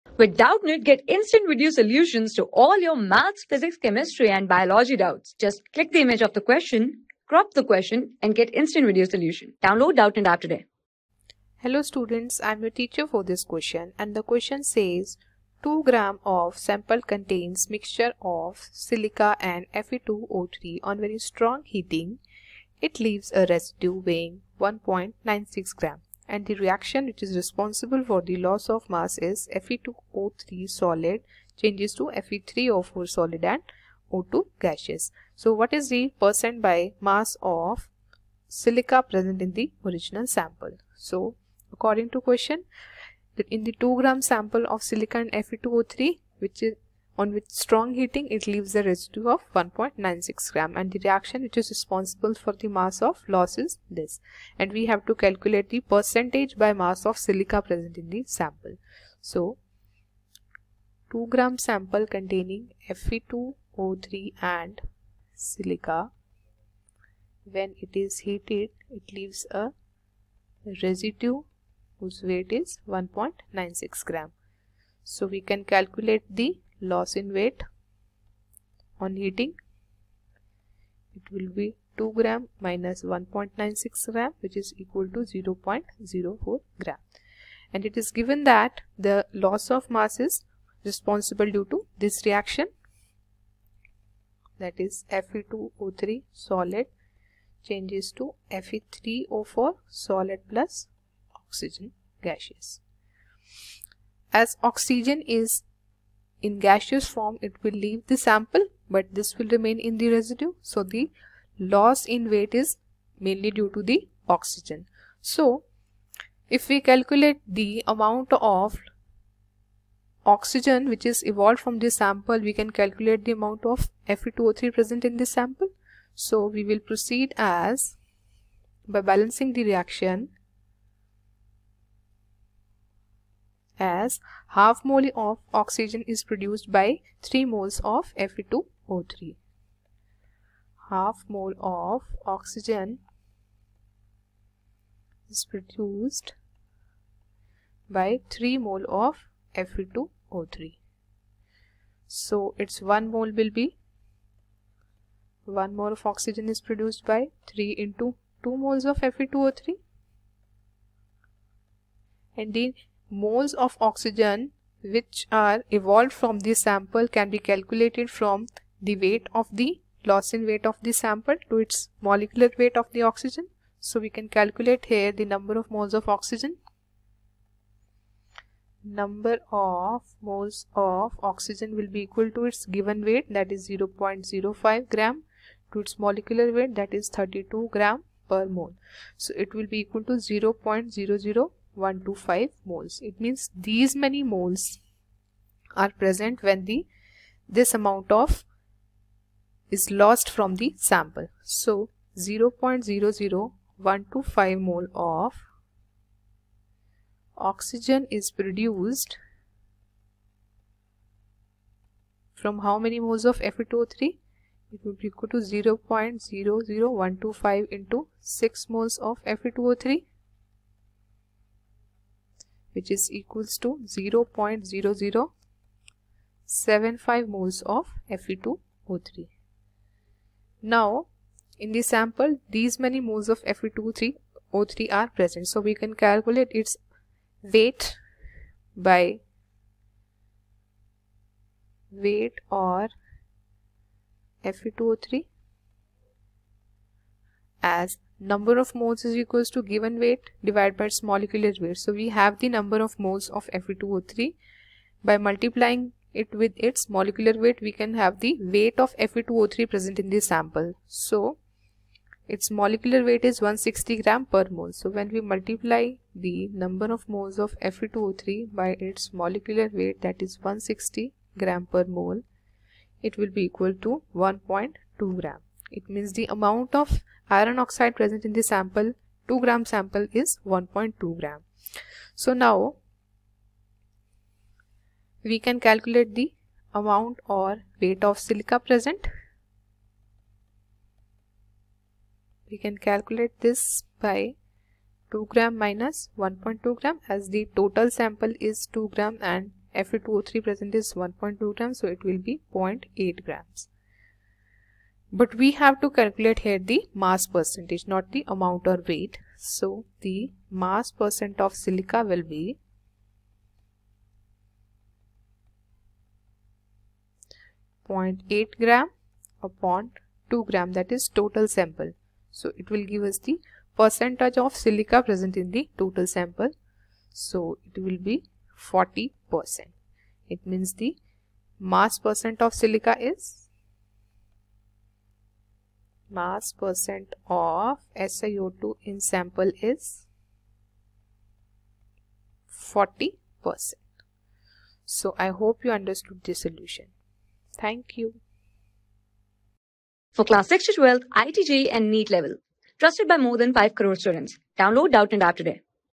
2.0g of a sample contains mixture of SiO_2 and Fe_2O_3. On very strong heating, it leaves a resi...
2.0g of a sample contains mixture of SiO_2 and Fe_2O_3. On very strong heating, it leaves a residue weighing 1.96 g. The reaction responsible for loss of mass is given below. Fe_2O_3(s)rarrFe_3o_4(s)+O_2(s), (unbalance equation) . What is thepercent by mass of SiO_2 in original sample ?
Class: 11
Subject: CHEMISTRY
Chapter: STOICHIOMETRY
Board:IIT JEE

👉 Have Any Query? Ask Us.